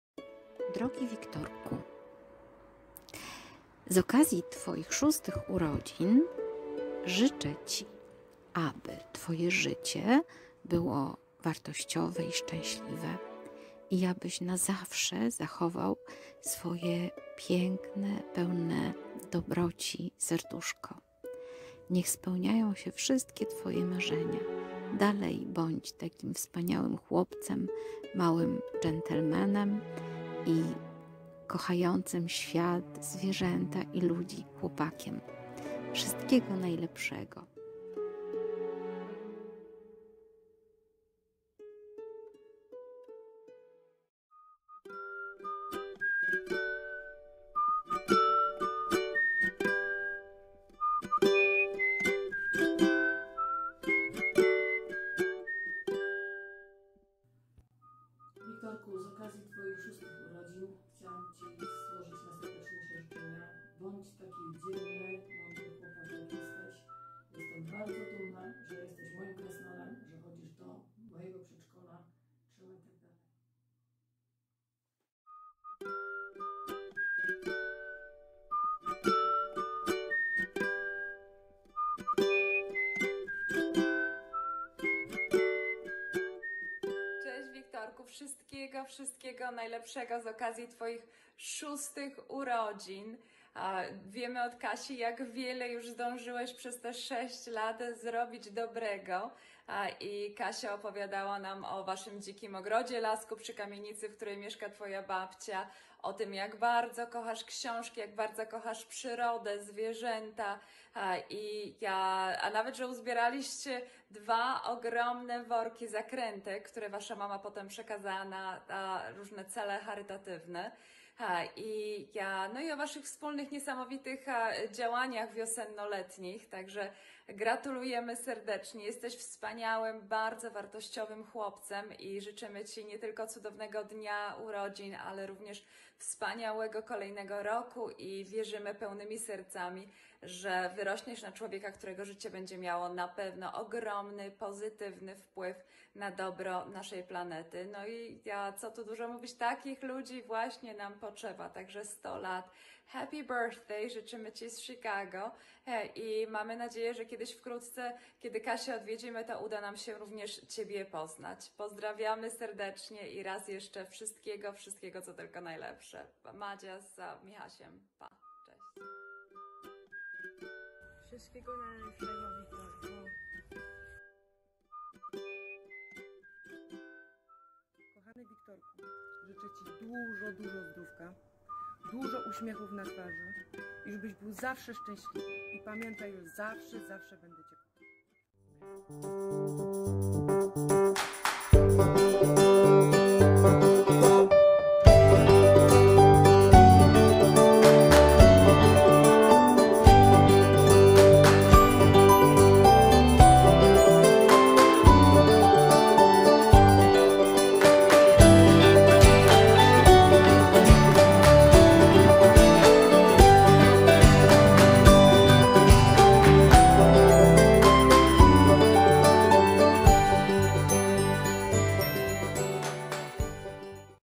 ŻYCZENIA URODZINOWE DLA WIKTORKA - NaszaPodwórkowaTelewizja 7
Wiktorek to przedszkolak, który właśnie kończy 6 lat.
Młody przyrodnik i ekolog, a także chłopiec o wielkim sercu, który chętnie angażuje się w pomoc dla innych.

W czerwcu wspólnie wysiewaliśmy kwiatki, aby ozdobić nasze podwórko.
Uczyliśmy się rozpoznawać gatunki drzew, obserwowaliśmy życie ślimaków, pomagaliśmy też ptakom i pszczołom, przygotowując dla nich poidełka i karmniki.
Od sierpnia organizujemy też wśród sąsiadów akcję zbierania nakrętek dla chorej Sandry: „Wkręć się w pomaganie!”

NaszaPodwórkowaTelewizja 7